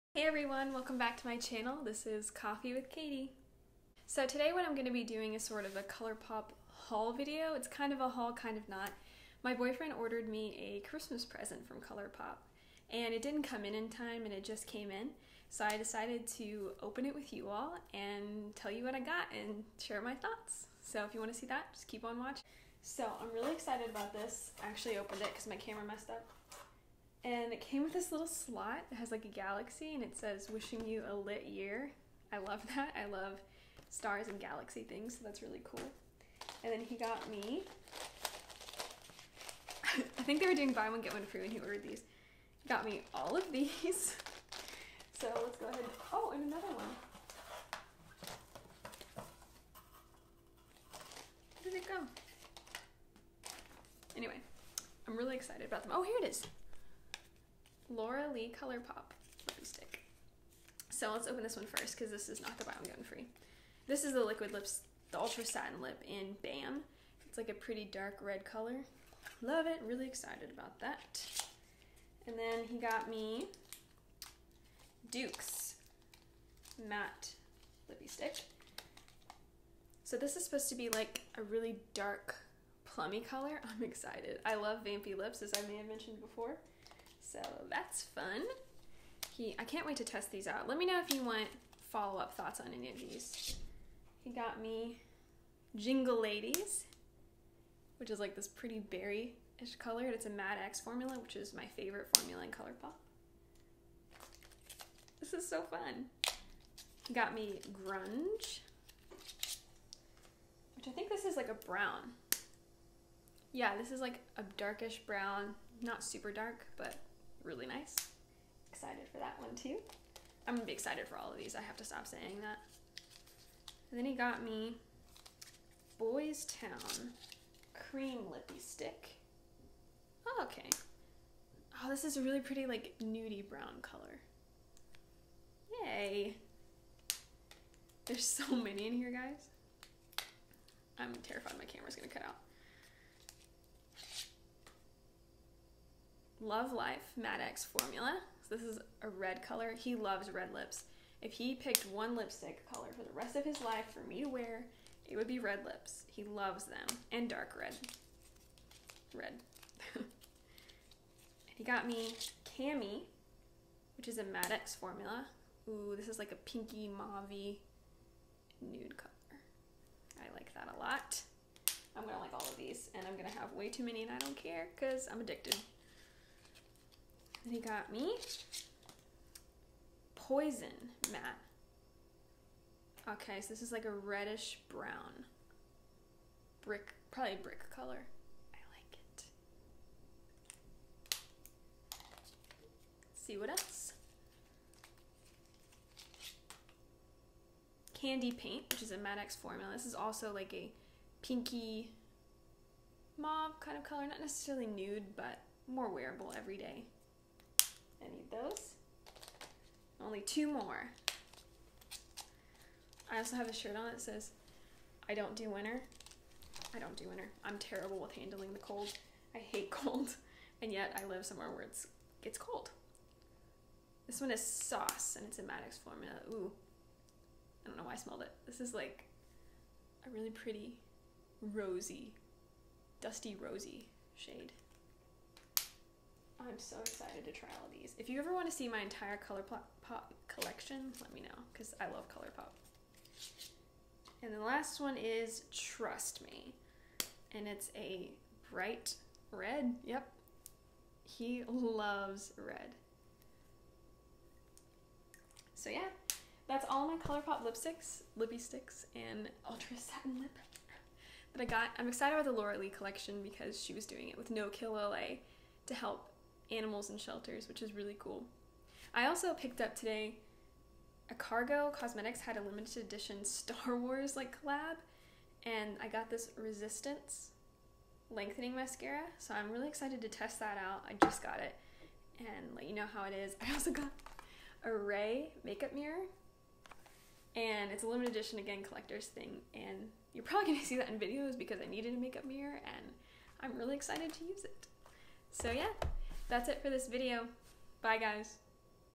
Colourpop Haul!
This is a colourpop haul! Sort of... my boyfriend ordered me some things from colourpop for Christmas, but it came late. So I opened it all up and showed y’all everything I got! Plus a few more surprises at the end. And yes... I love lipsticks ❤️